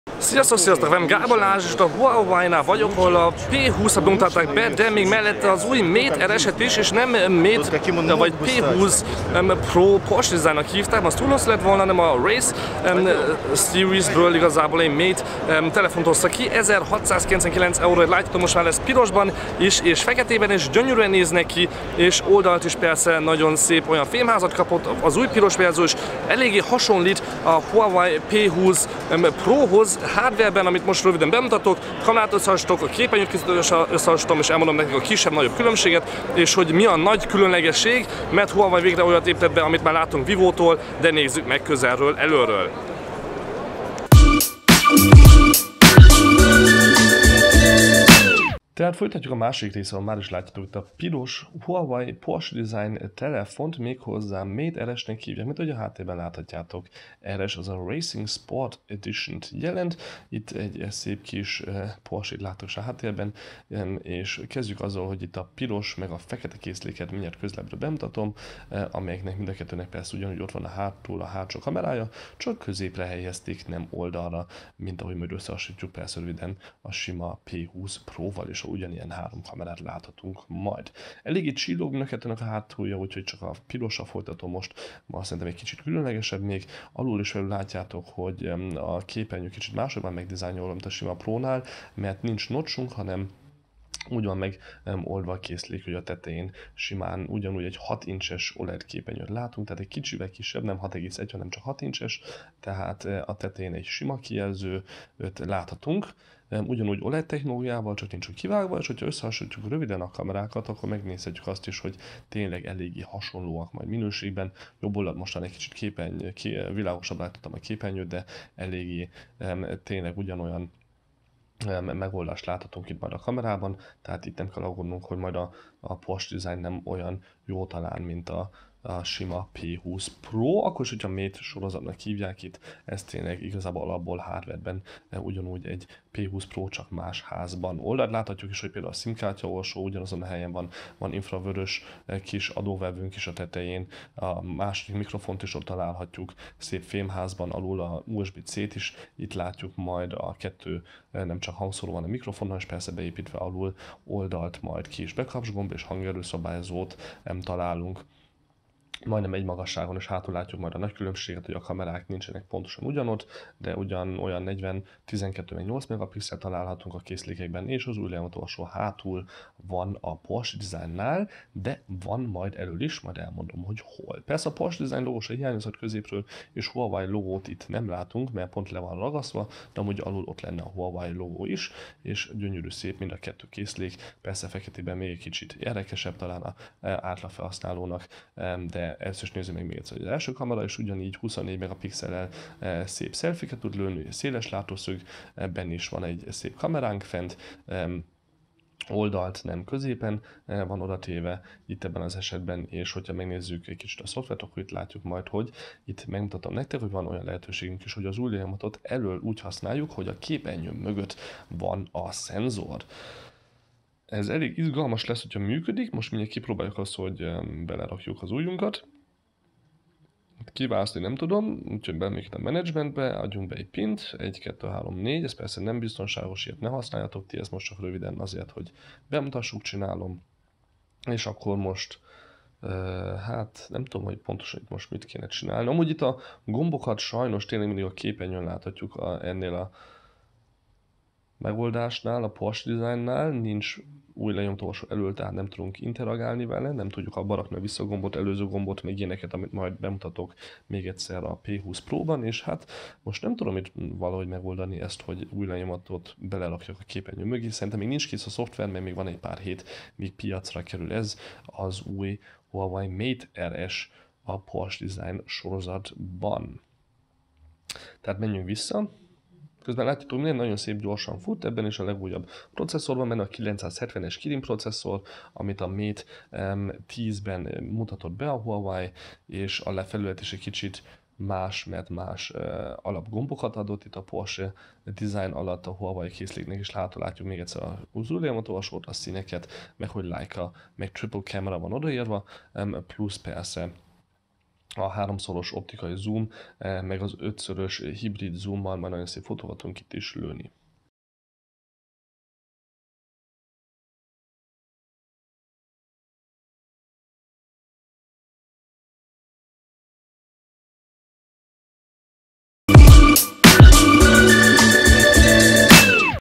Képernyő mögötti Újlenyomatolvasó és 500.000 Forint | Huawei Mate RS Porsche Design bemutató videó
Csak melekletesen mutatták be az új Huawei P20 melett a Huawei Mate RS Porsche Design Okostelefont. Nem csak drágább ez a telefon hanem van benne egy kettö olyan technológia ami-t másik Huawei Okostelefonban még nem láttunk.

Itt röviden bemutatom nektek a 1699 Euros Okostelefon-t.

Ezért a videóért nem lettem fizetve és csak az én véleményemet tartalmazza. Más helyzetben itt elolvashatjátok hogy milyen mértékben dolgoztam együtt a céggel. A Terméket teszt után vissza kell küldeni.